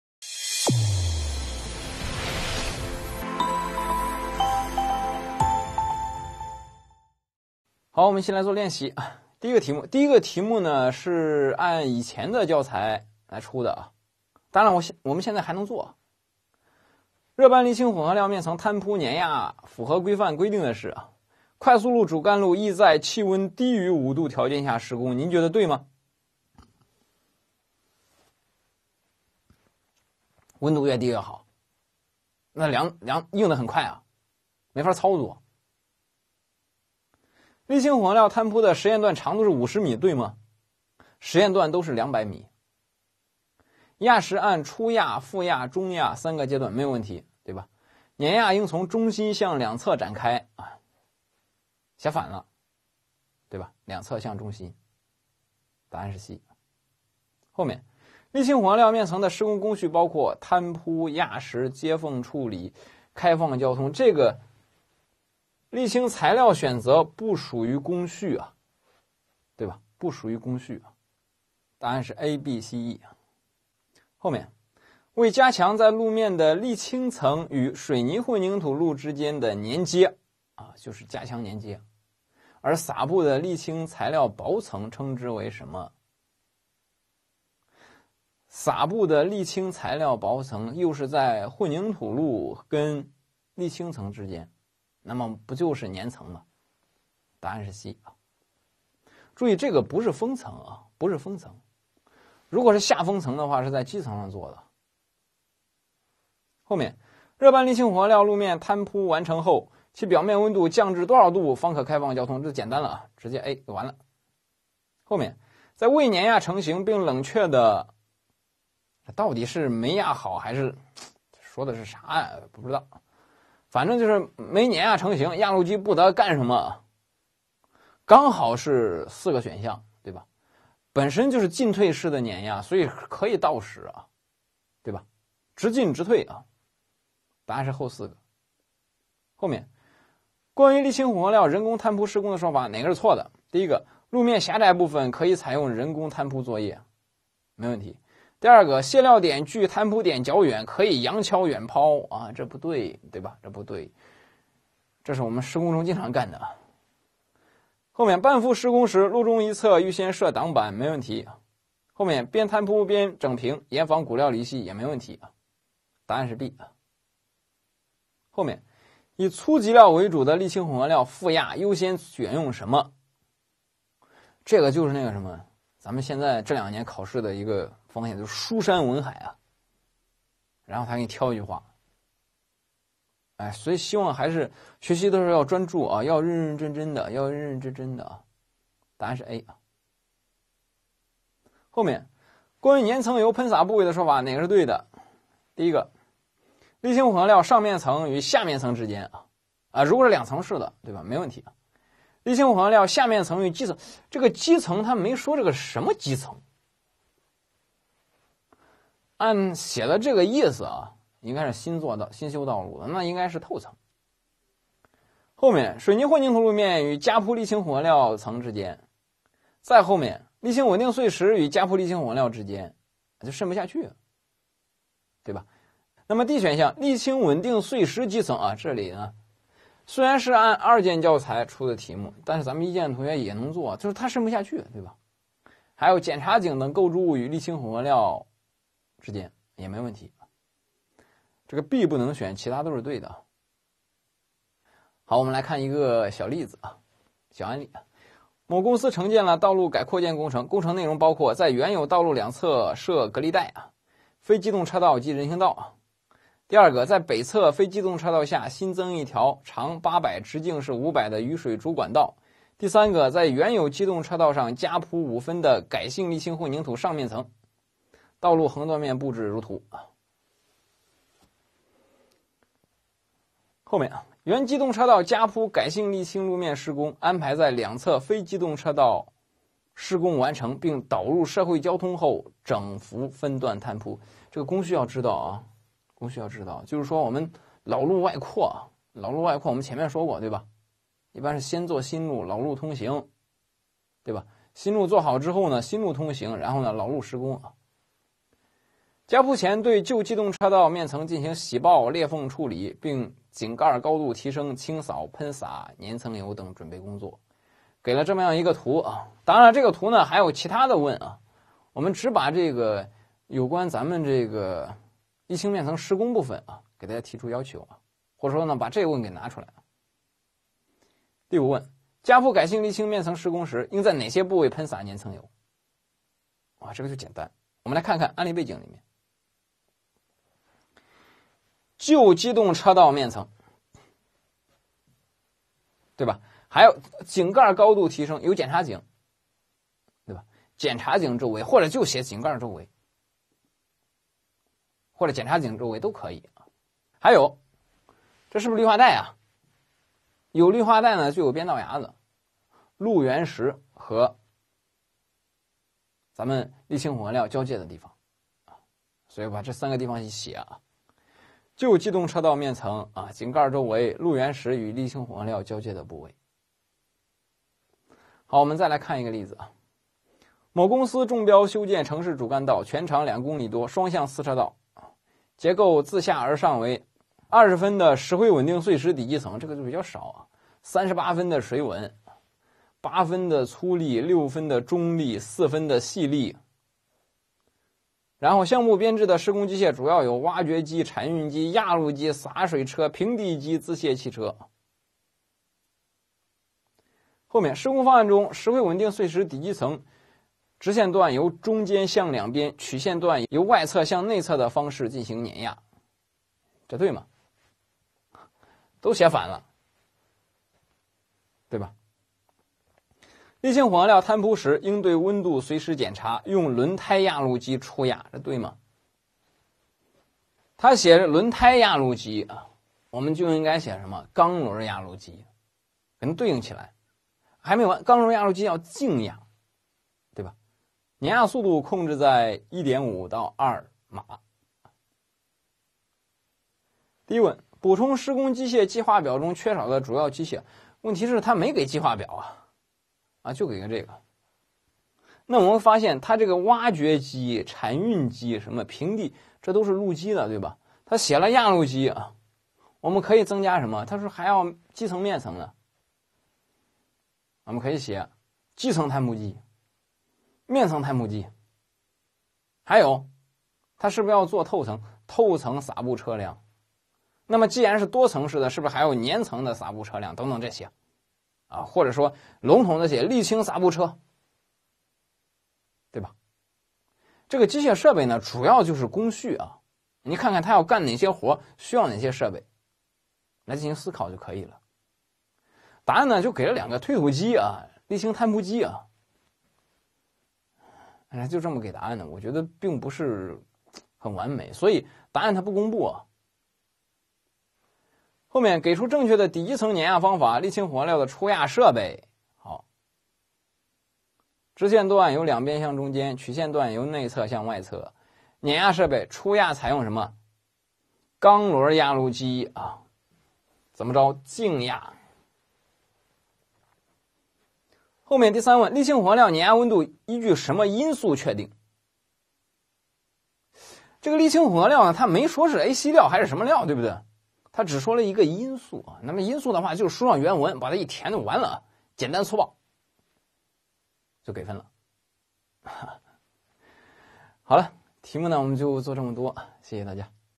2022 一级建造师 市政 曹铭明 习题班习题课第11讲 1K411041沥青混合料面层施工技术
2022 一级建造师 市政 曹铭明 习题班